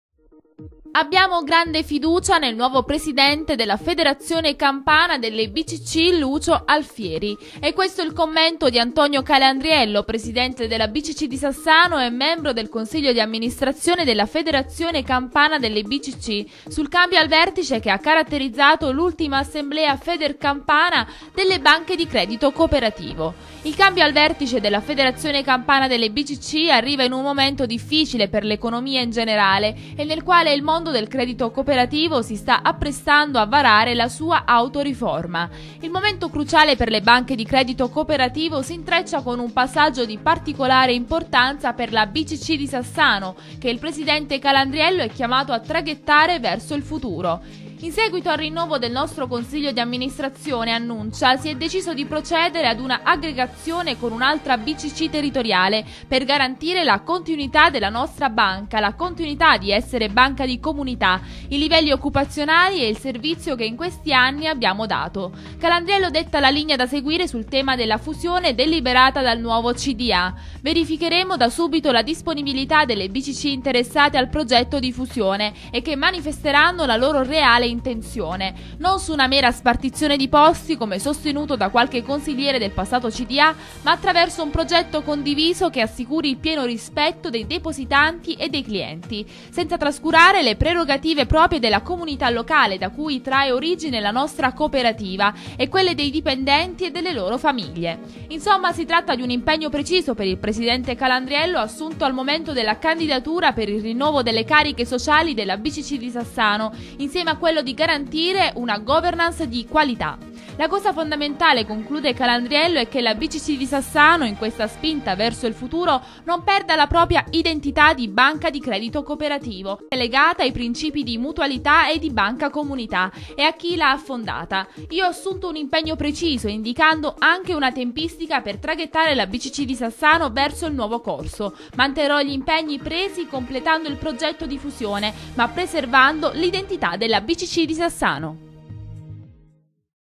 Bcc di Sassano, il Presidente Calandriello detta la linea per il prossimo futuro
Bcc di Sassano, il Presidente Calandriello detta la linea per il prossimo futuro 09/07/2015